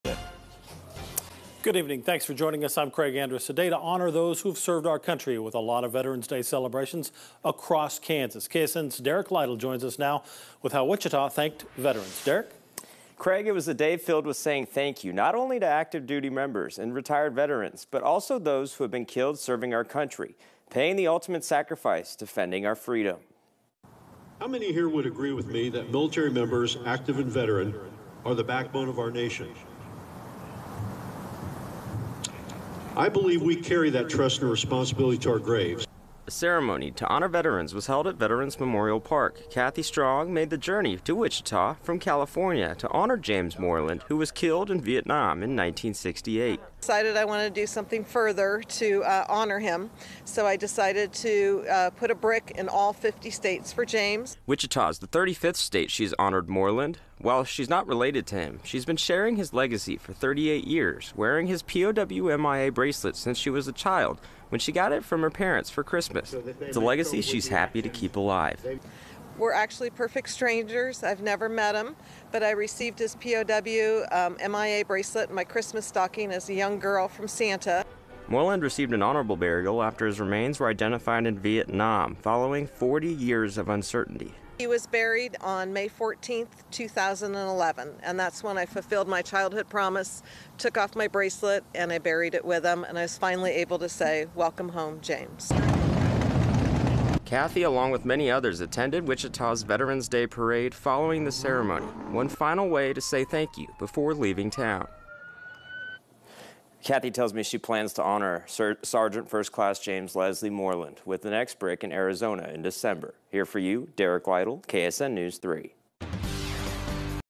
Woman finds special way to honor a fallen soldier on Veterans Day
A day to honor those who have served our country. A lot of Veterans Day celebrations take place across the country on Saturday. Many came out to say thank you to not only active duty members and retired veterans, but also those who have been killed serving our country.